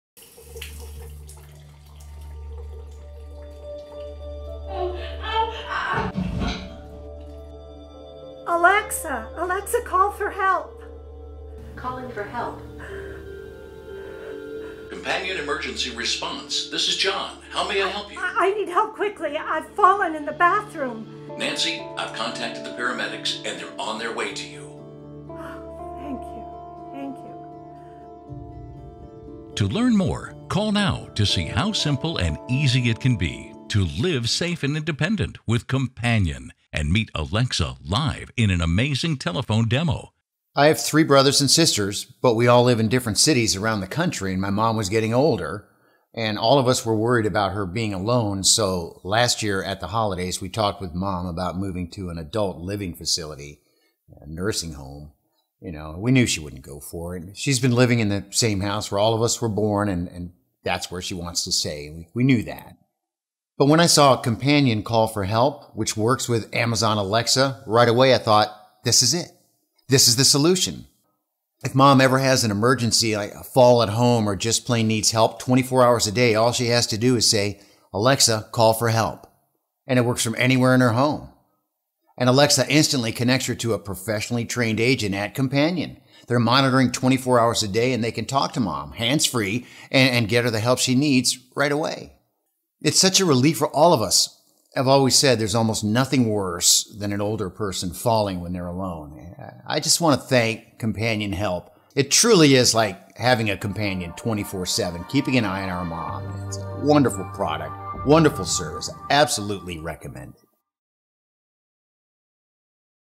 What happens if you slip and fall in the shower or bath? Watch...
1 in 4 People over 65 Fall Each Year -- The results can be life threatening.  Watch this Live Bathroom Slip & Fall turn from potential disaster to an Amazing Heroic Ending for this Family.